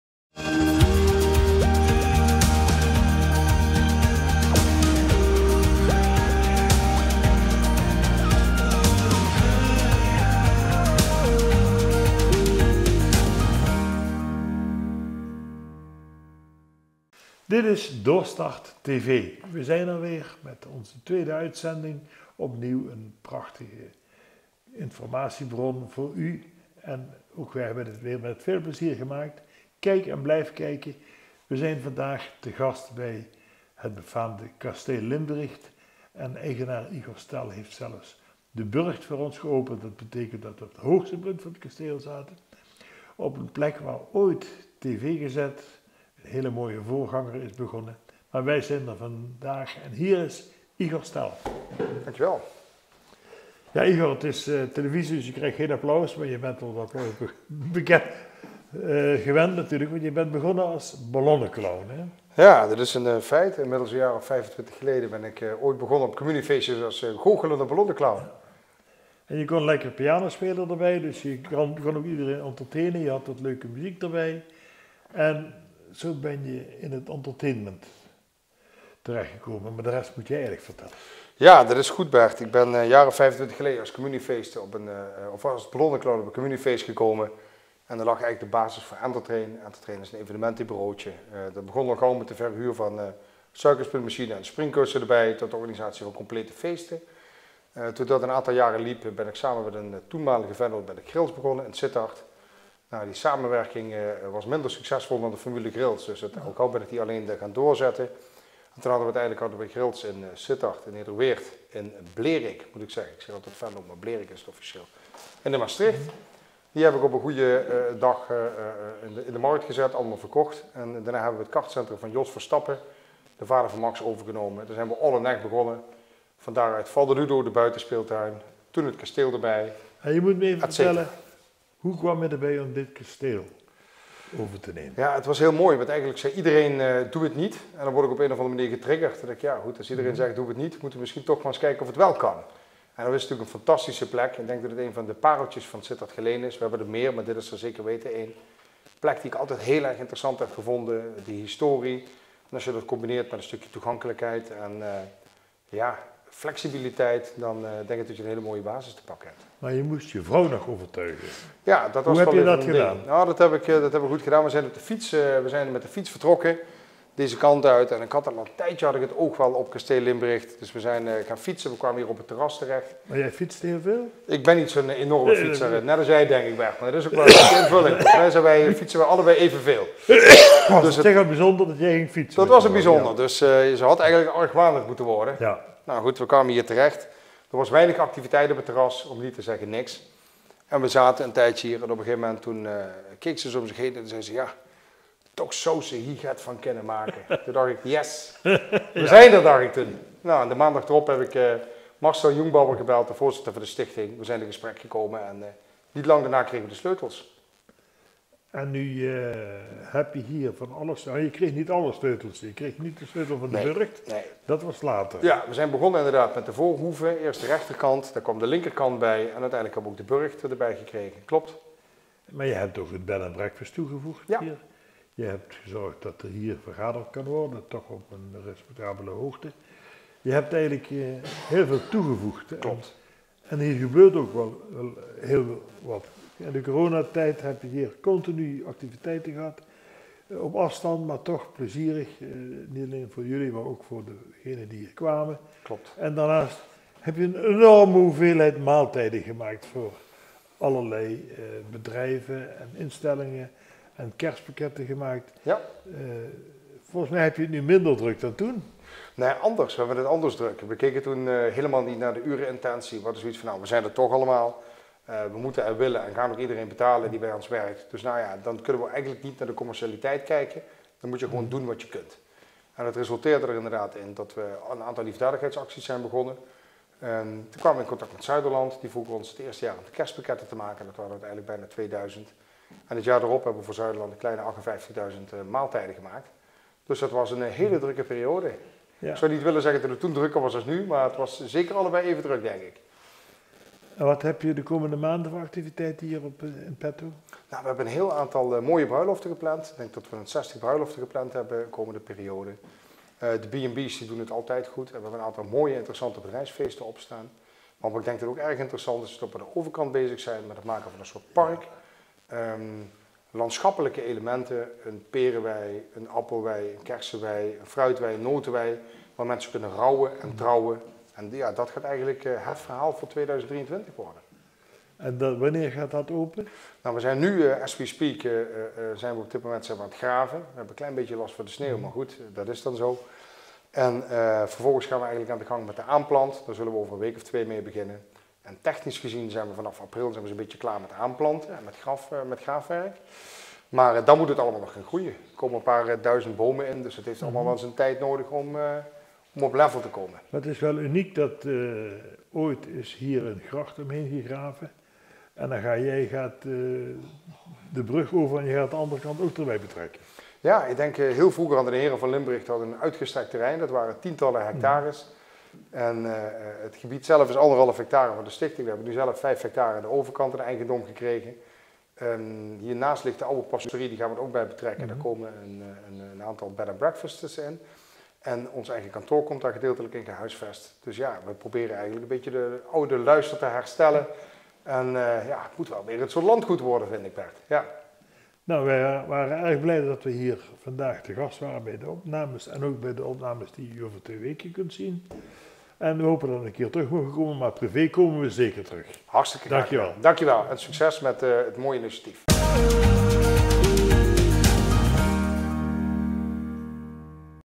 Studio 046 Igor Stel
Igor Stel begon zijn creatieve leven als ballonnenclown en goochelaar met een brede muzikale basis. Hij zette entertainmenthardwareverhuurder Entertrain op, stapte in de horeca en runt momenteel onder andere All-in- Echt, Valdeludo in Echt, Kasteel Limbricht en De Hompesche Molen in Stevensweert. En er is meer. Hij vertelt het allemaal in deze reportage.